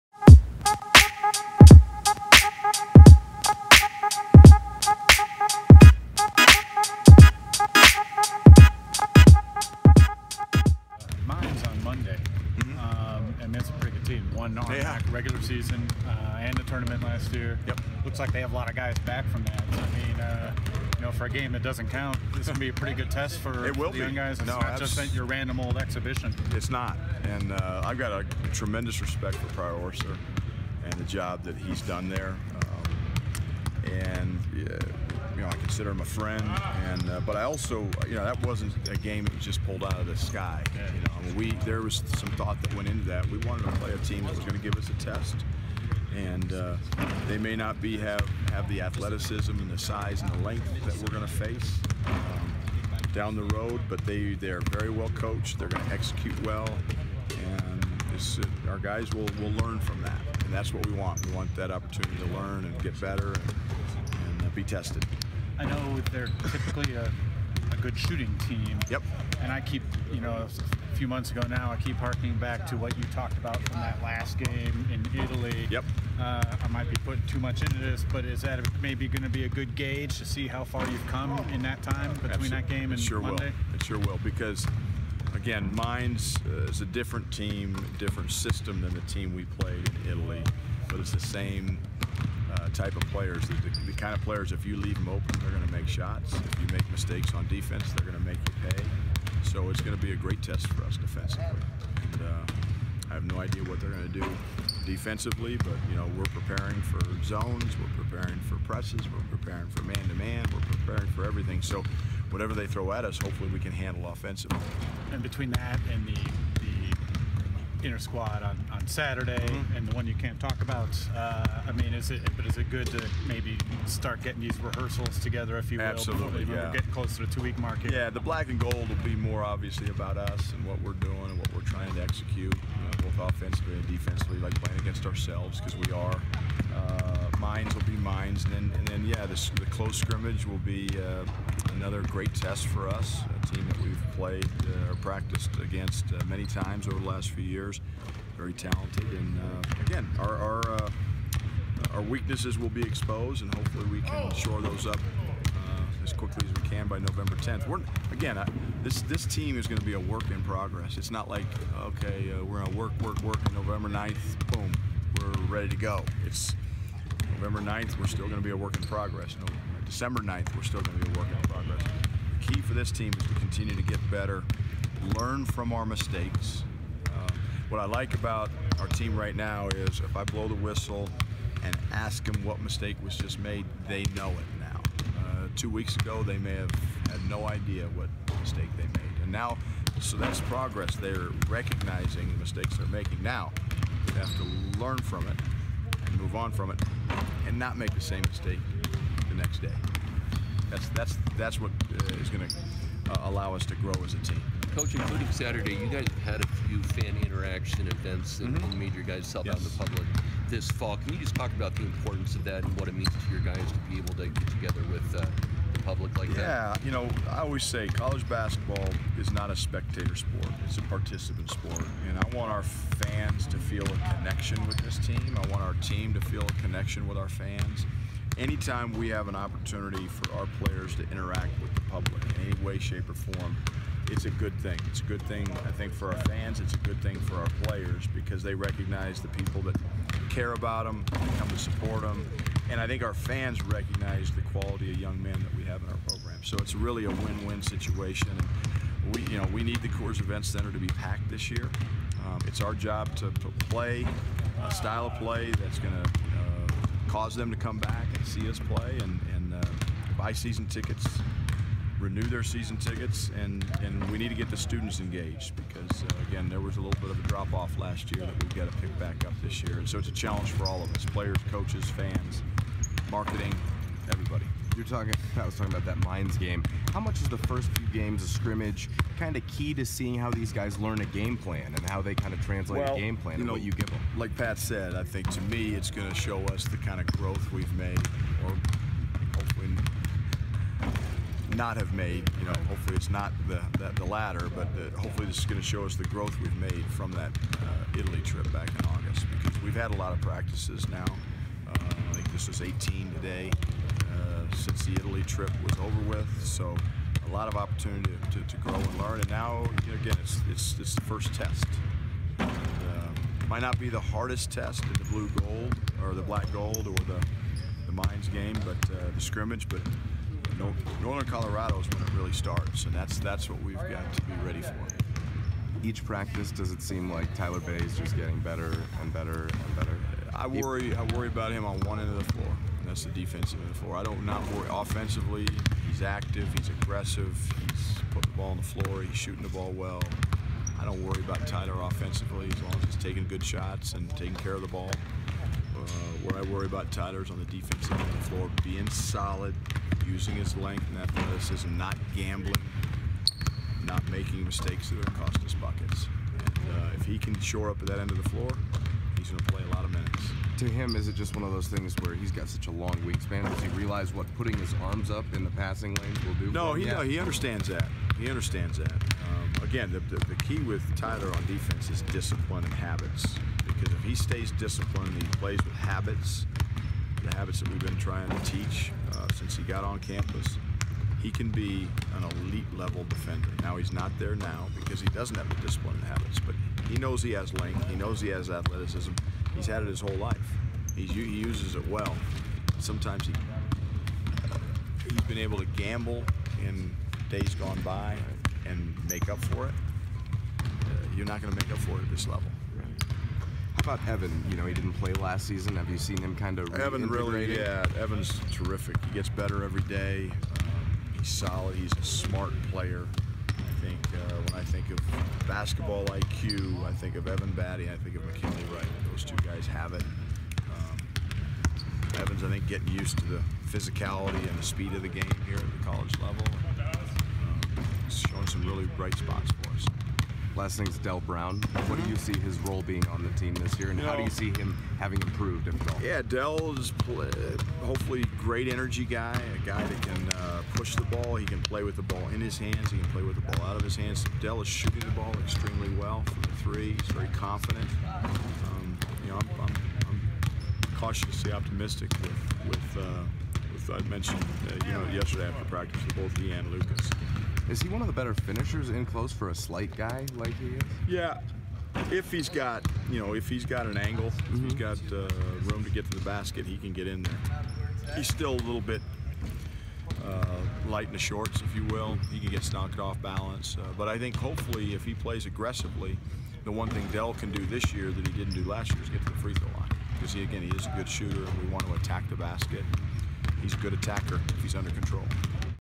PRACTICE REPORT: Head Coach Tad Boyle (10/24/17)
Colorado Buffaloes Head Coach Tad Boyle addressed the media after practice on Tuesday, October 24th, 2017.

Coach Boyle addresses the Buffs' upcoming exhibition game at Colorado School of Mines on Monday, October 30th, 2017. It will be the first test of competition against an opposing team since CU's preseason trip to Italy.

Tad also talks about addressing the attendance issue at the Coors Event Center and says it will take a complete effort from the program. However, he acknowledges that Colorado will have to put a product on the floor that will draw fans.

Tad talks about what he's seen from freshman forward Tyler Bey and where he is at with his development. Bey has the most potential of anyone in the 2017 recruiting class. Boyle says Tyler has to focus on being consistent on the defensive end of the floor every day.